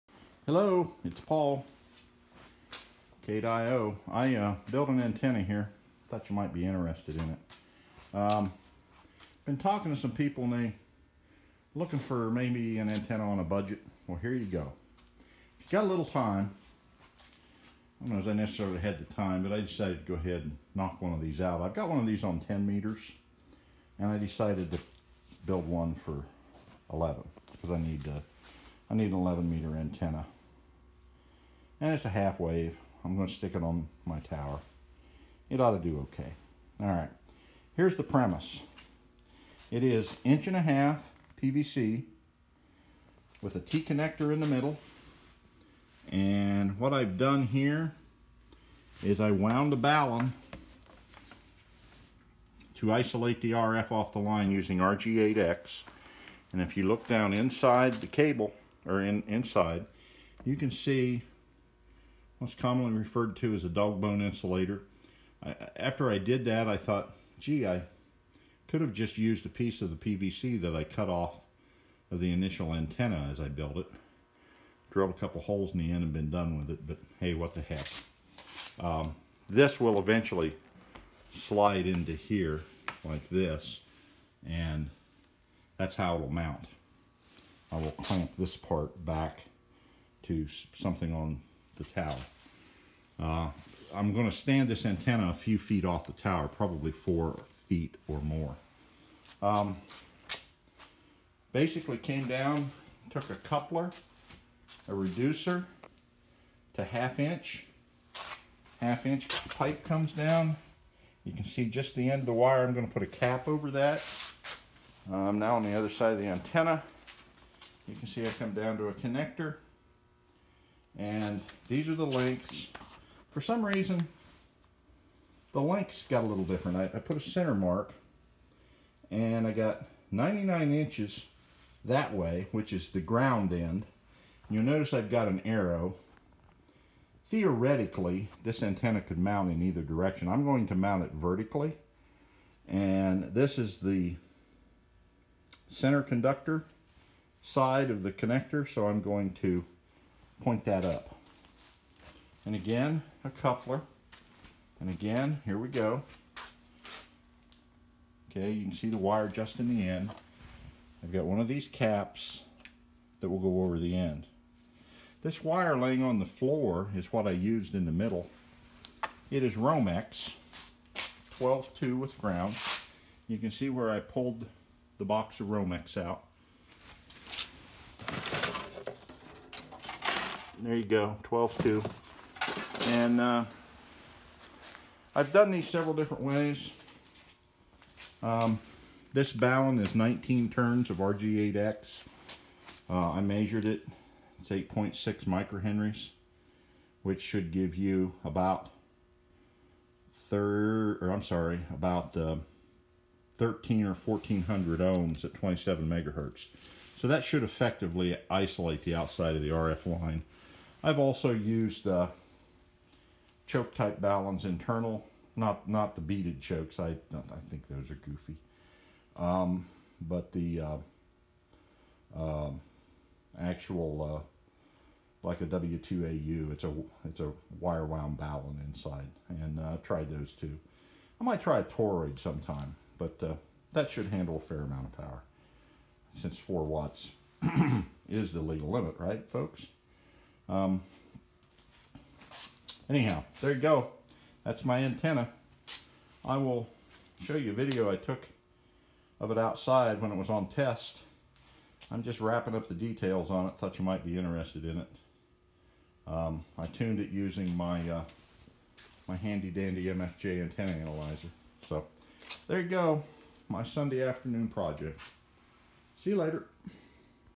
Cheap, homemade, cb and 10 meter antenna from pvc and Romex wire
I made this antenna a couple years ago and decided to finally make the video public. I have it mounted side mount on the tower and by sliding it to and from the tower you can affect the tuning dramatically. I get really good performance from this antenna and it is up about 25 feet it would be quite easy to run this either horizontal or vertical and use larger coacts for higher power operation.